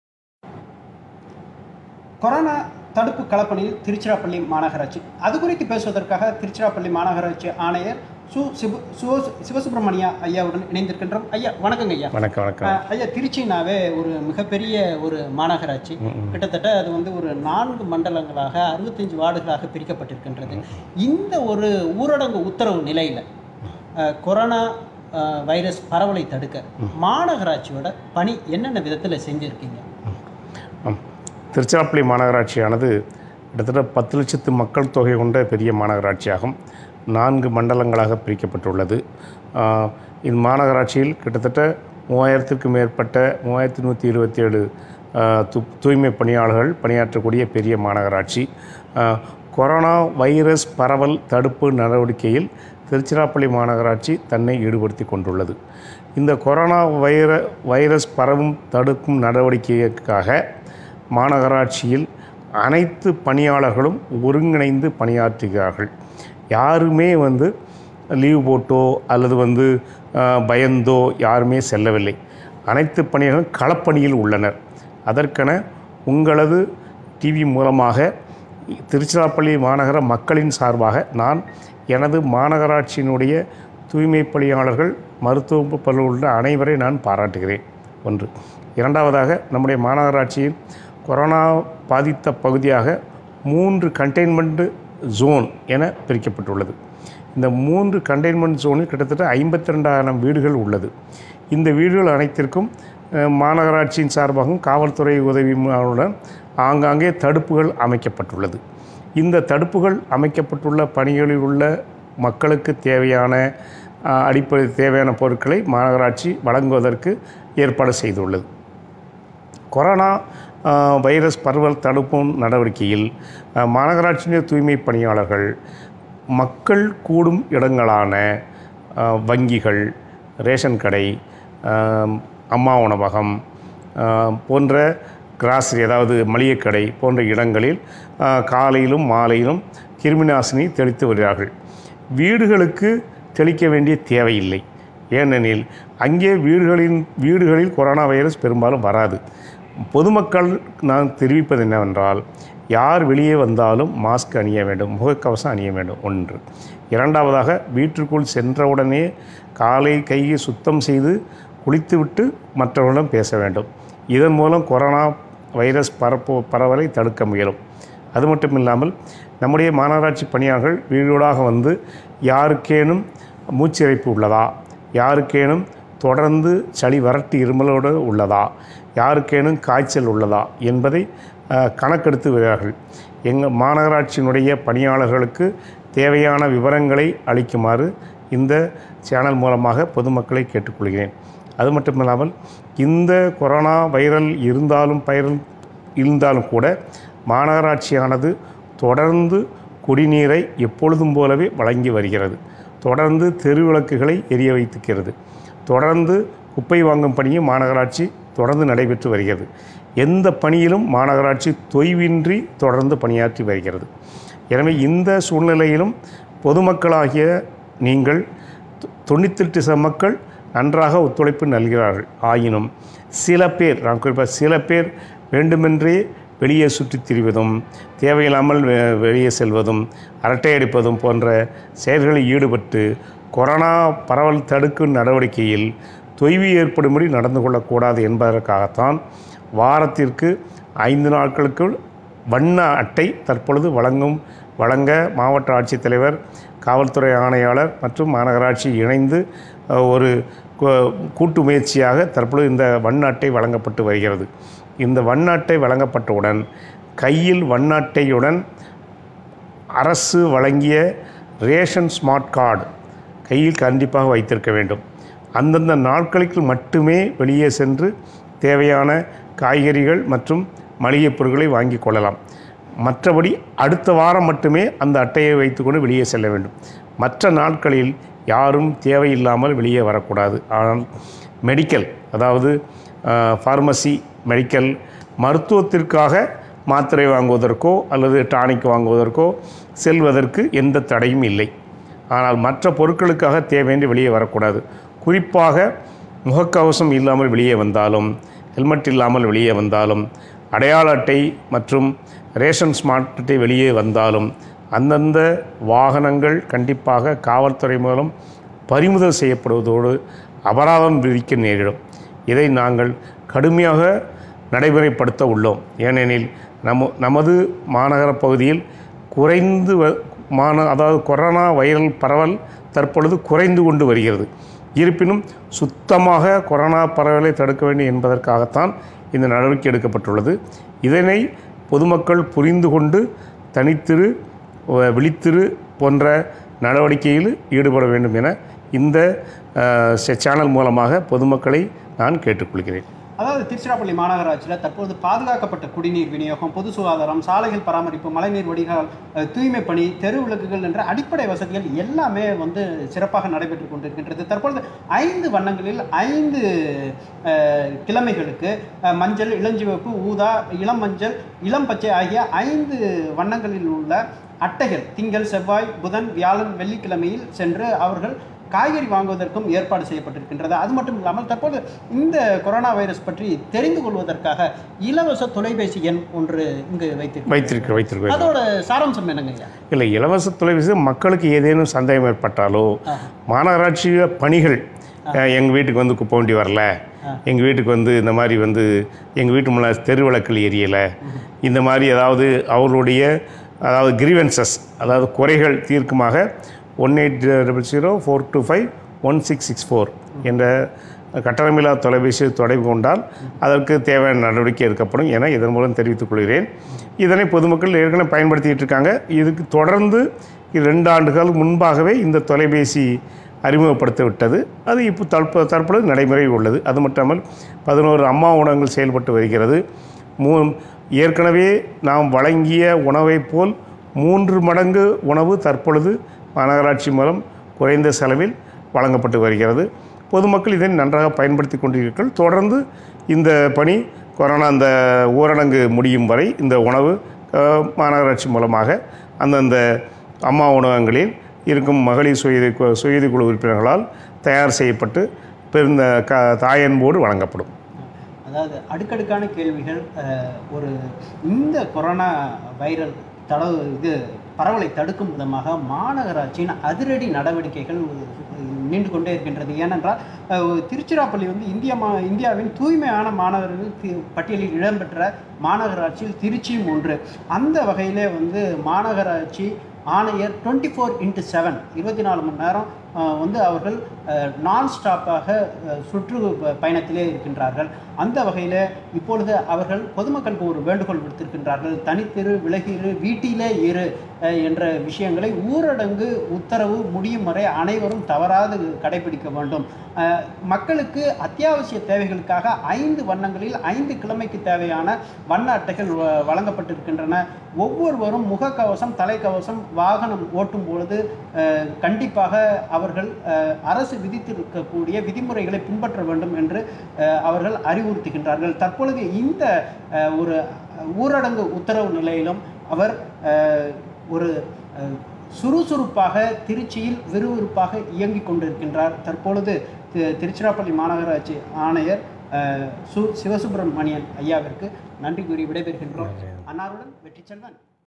திருச்சி மாநகராட்சி ஆணையர் - சிவசுப்ரமணியம்
கொரோனா களப்பணியில் திருச்சி மாநகராட்சி ஆணையர் சிவசுப்பிரமணியம் உடன் நேர்காணல் Ntrichy.com ஆசிரியர் வெற்றிச்செல்வன்